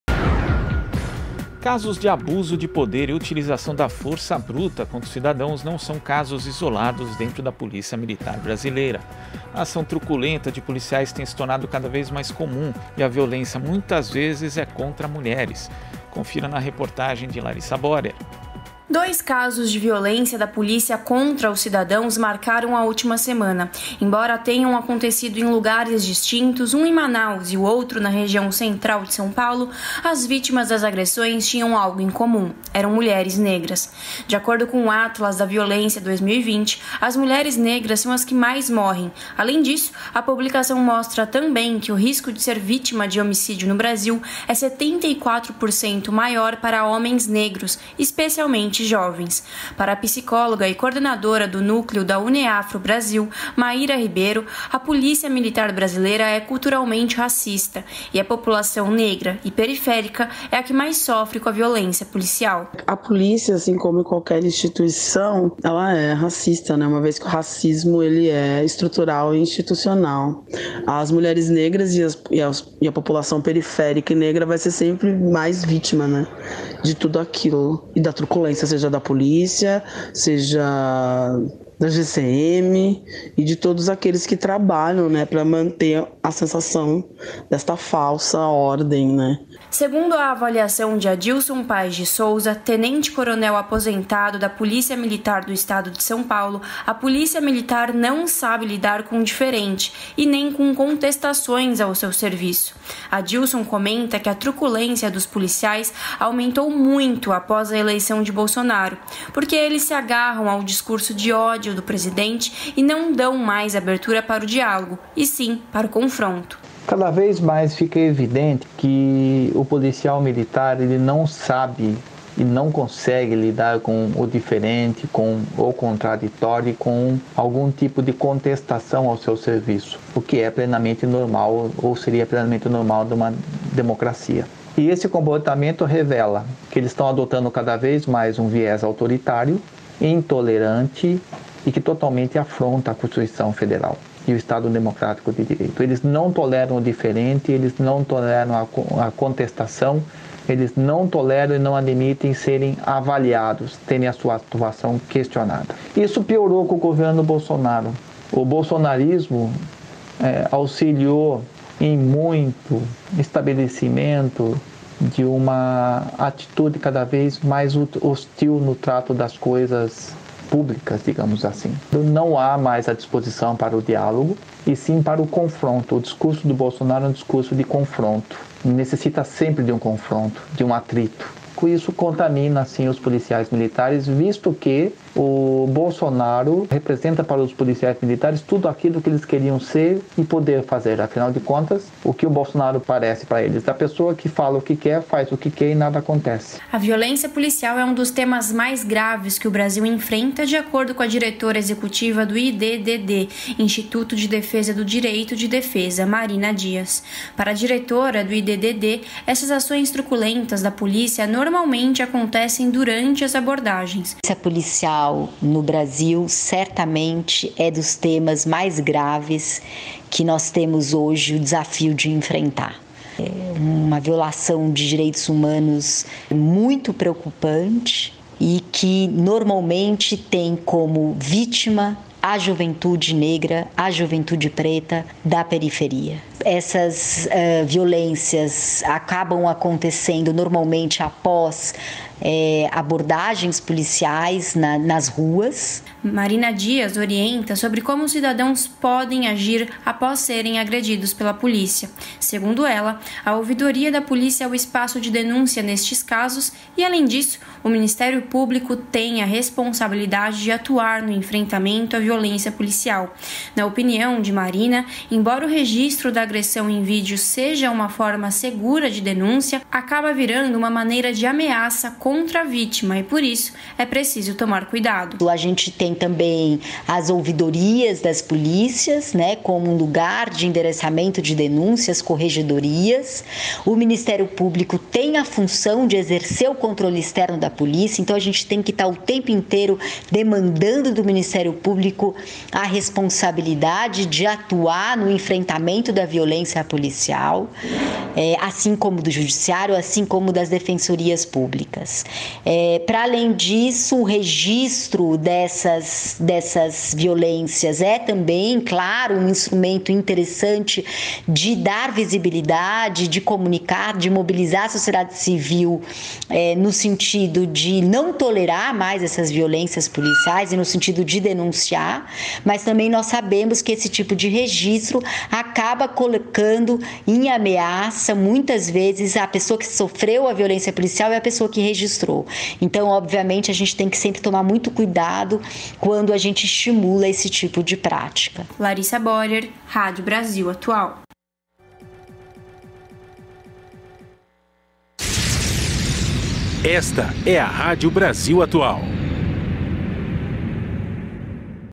A violência gratuita da Polícia Militar contra a população 🎙
Casos de abuso de poder e a utilização da força bruta contra os cidadãos não são casos isolados dentro da polícia militar brasileira.
A ação truculenta de policiais tem se tornado cada vez mais comum e a violência, muitas vezes, é contra mulheres.

Reportagem de Larissa Bohrer